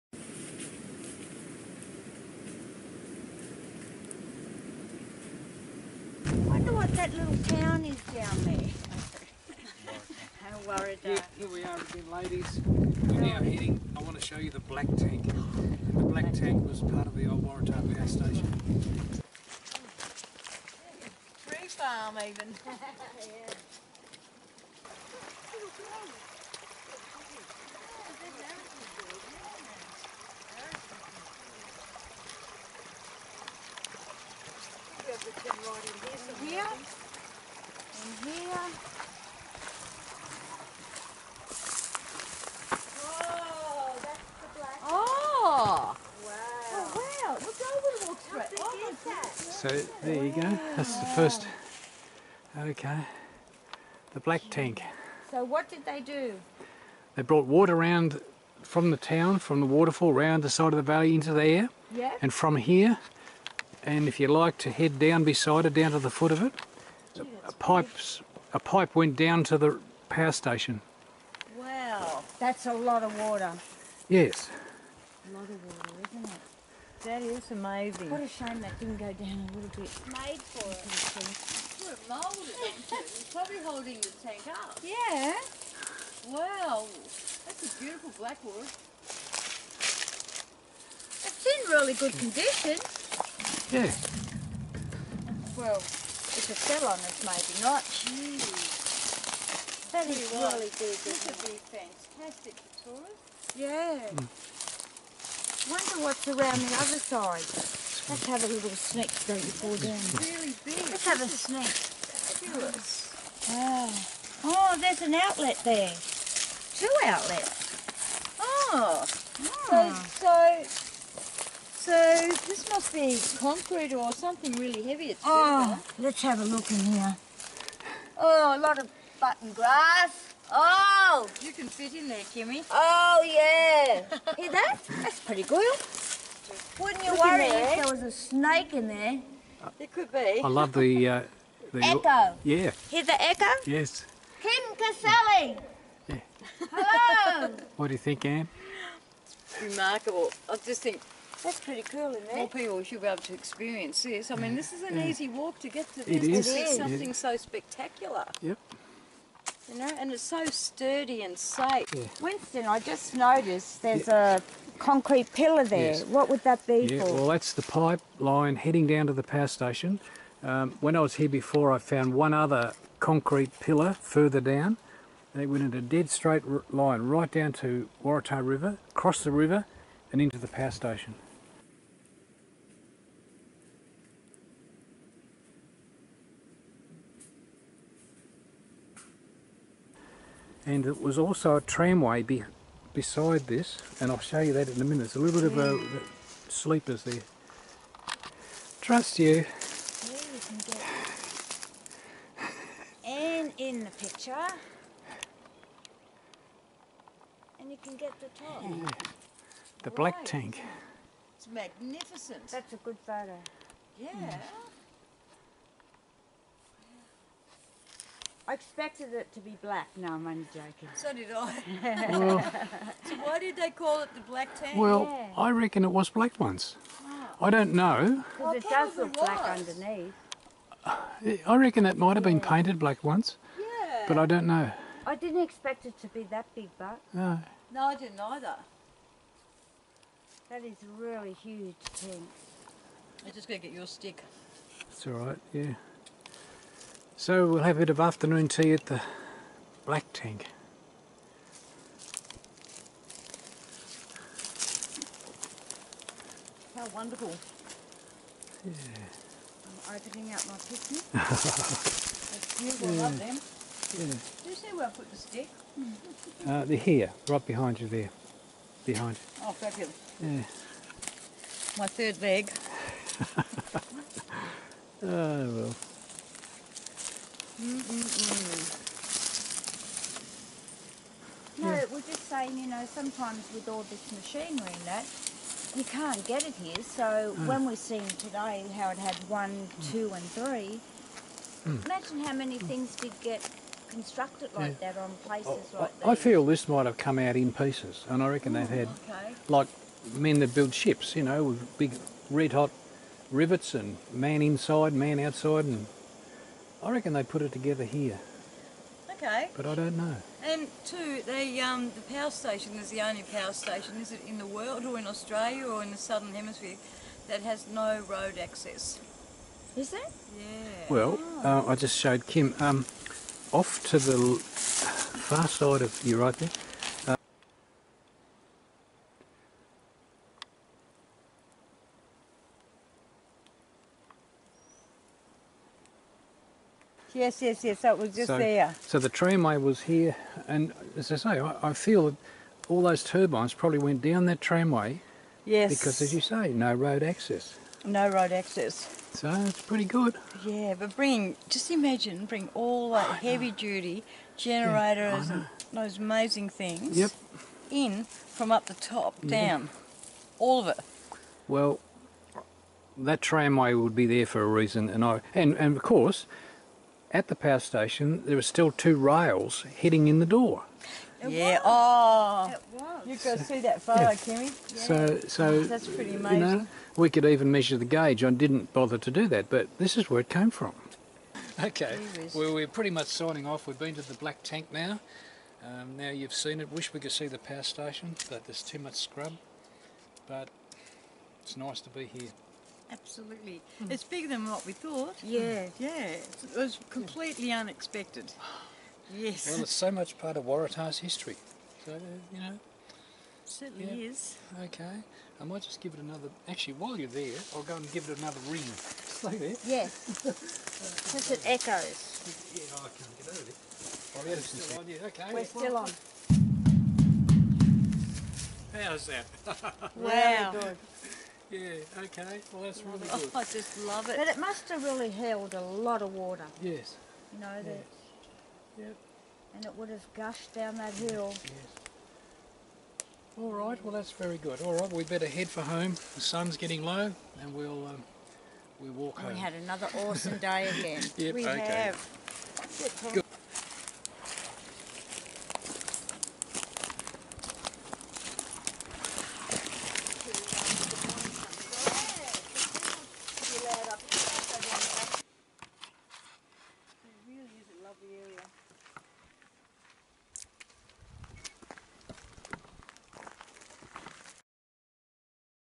Black Tank Anne Kim
A walk to the Black Tank with Anne Dunham and Kim Kecely.